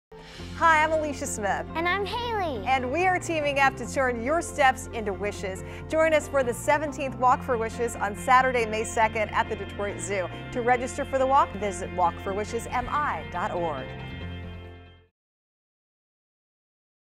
Walk for Wishes® - Southeast Michigan 2015
Join Make-A-Wish® Michigan and WXYZ anchor Alicia Smith for the 17th Annual Walk For Wishes® - Southeast Michigan on May 2, 2015 at the Detroit Zoo in Royal Oak.

In 2014, walkers in Southeast Michigan helped raise more than $400,000 to help grant the wishes of Michigan children with life-threatening medical conditions, making it the largest grossing Make-A-Wish walk in the country. Join us May 2 and help us keep that record!